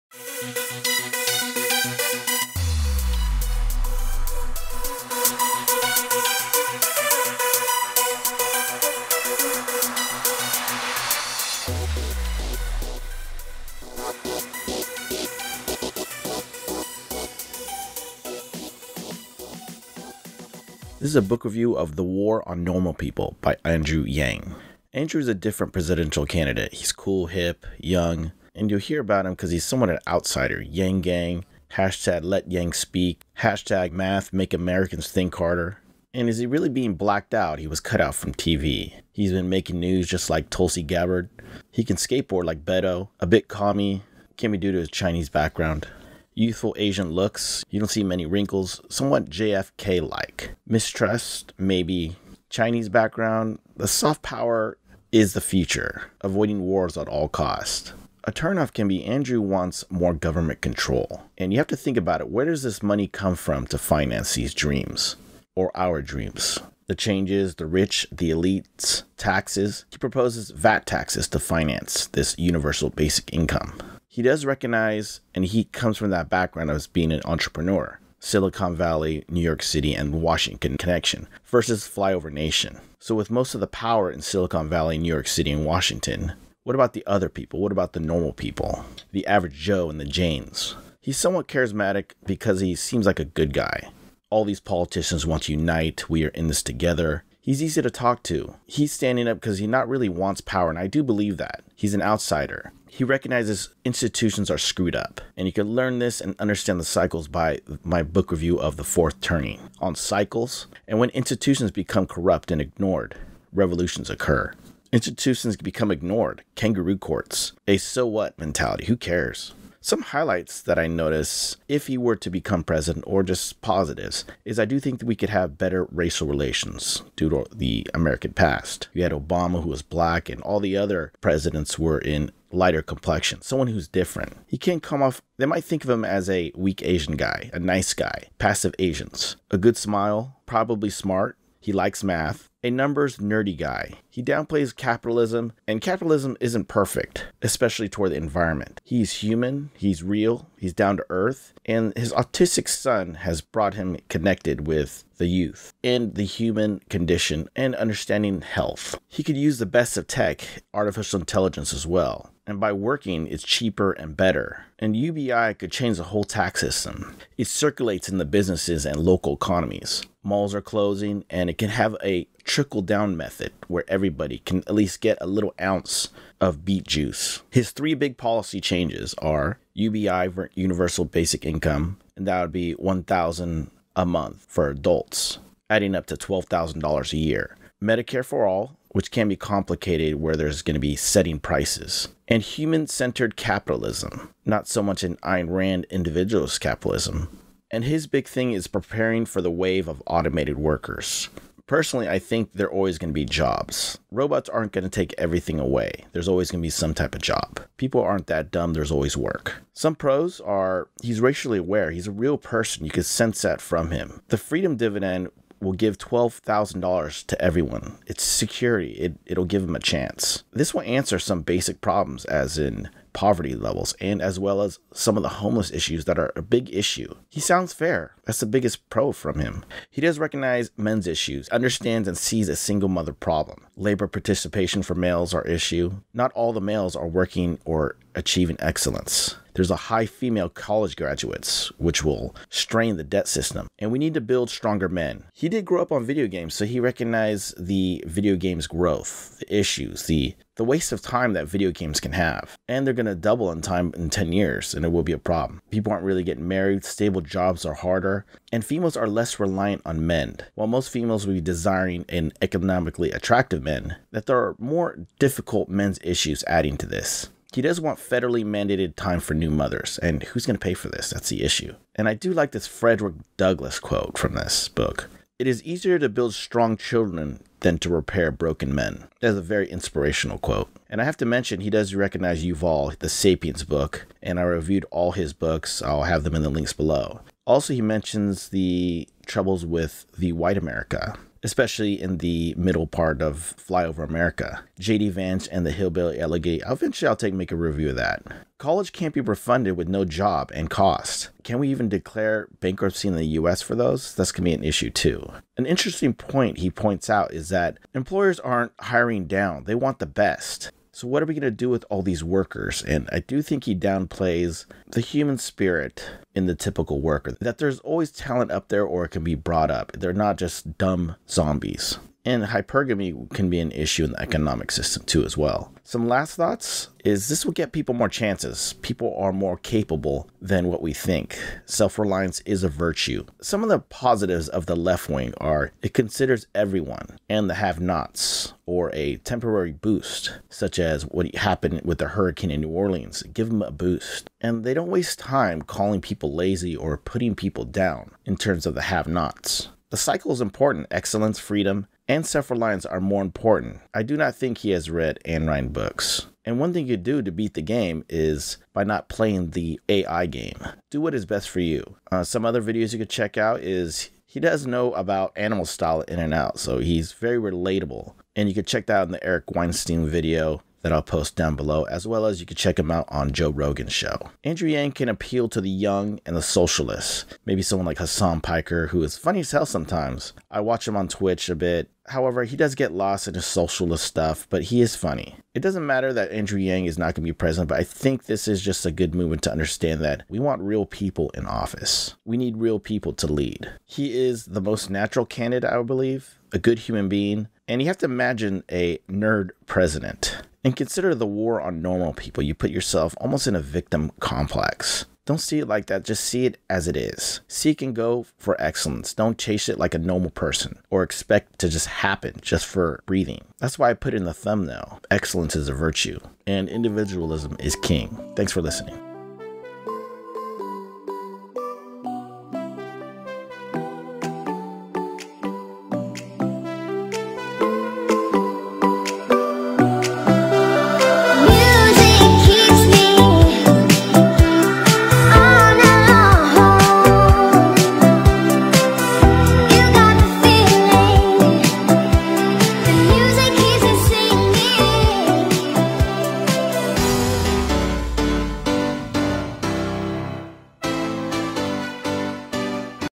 Andrew Yang - The War on Normal People - Book Review
Book Review of The War on Normal People: The Truth About America's Disappearing Jobs and Why Universal Basic Income Is Our Future by Andrew Yang.

Andrew is an interesting fella... human, wacky, yet real.  Learn why UBI is not functional and why excellence should be pursued.  Either way... join the Yang Gang. Is it the Silicon Valley/NY/Washington   VS   Flyover Nation?   Or do we all need a lift?

A great book to read to learn about a Presidential candidate who is actually human.  Institutions are going down in 2020.